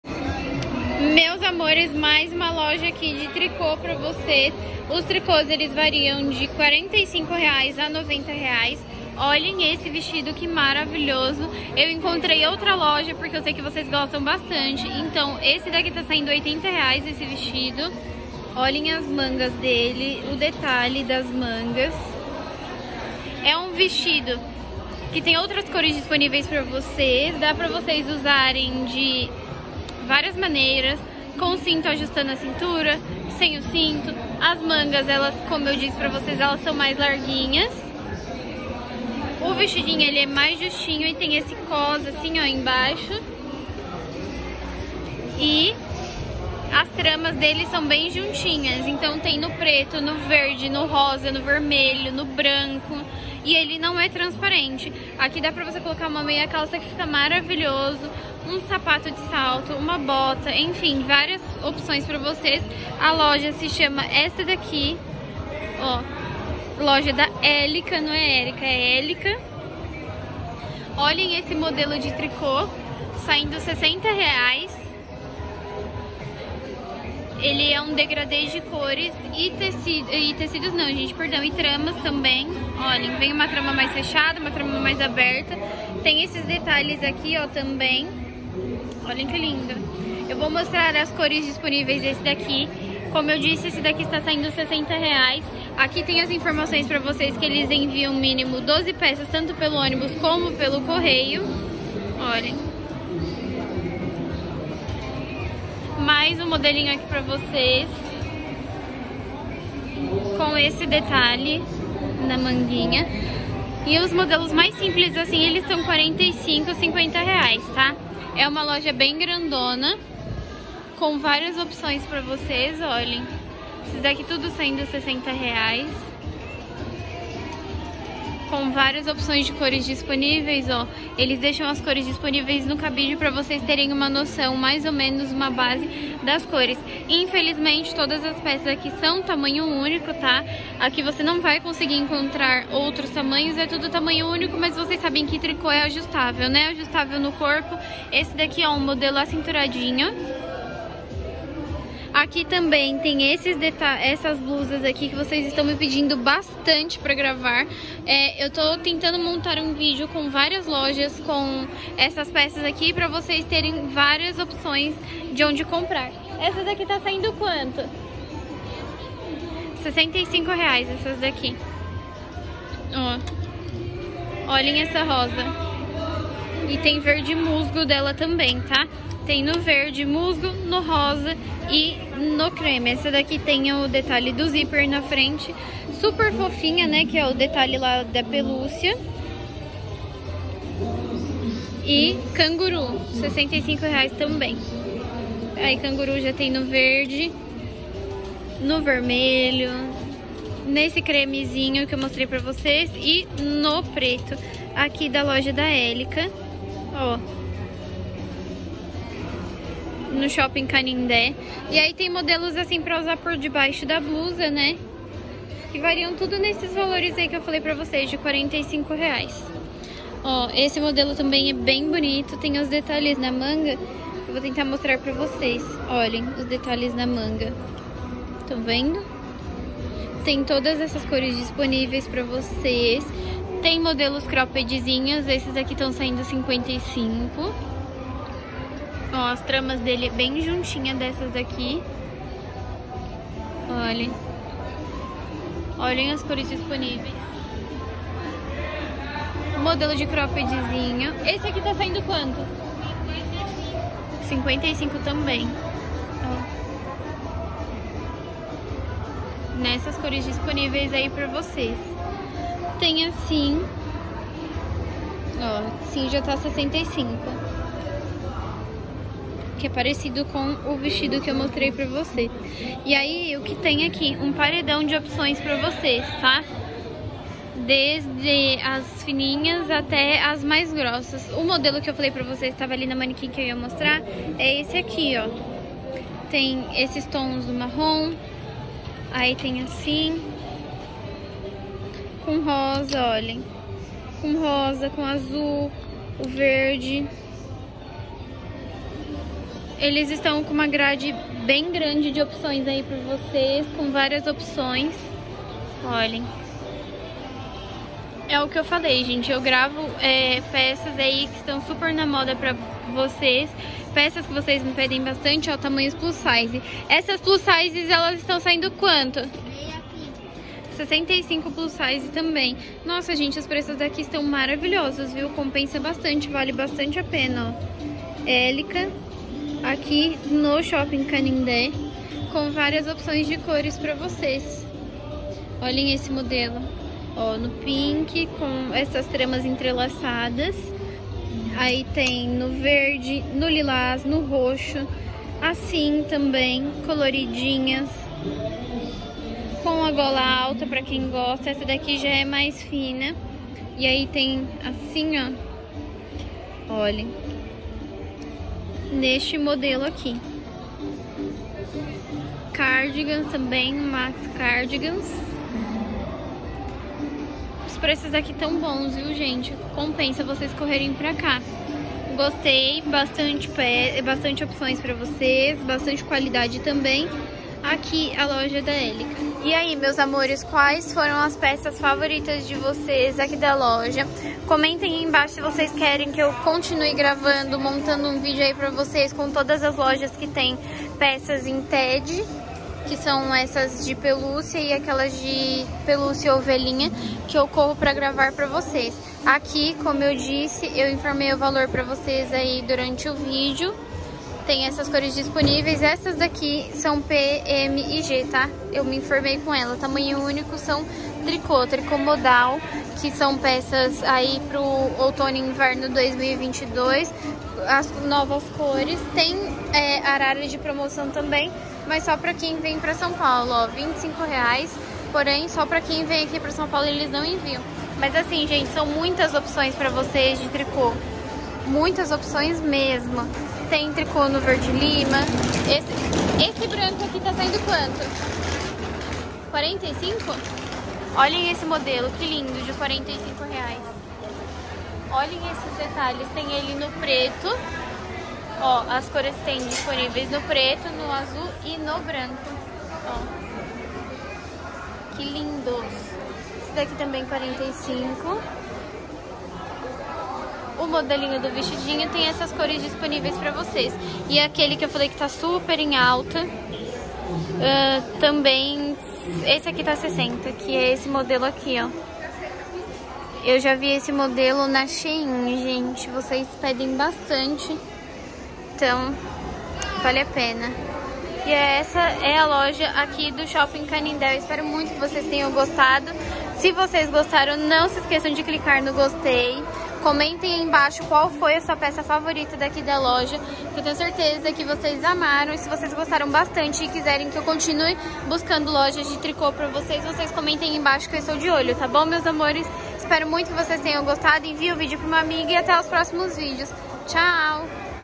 BRÁS - MODA OUTONO/INVERNO TRICOT EM BLUSAS ELEGANTES, LINDAS E ESTILOSAS NO SHOPPING CANINDE
Olá meus amores, o vídeo de hoje seguindo a linha de Tricot, hoje vou mostrar novos modelos que encontrei na loja da Elica, são modelos lindos, tem alguns elegantes e também estilosas, espero que gostem!

Câmera que uso: G7x Markll
Editor de vídeo: Filmora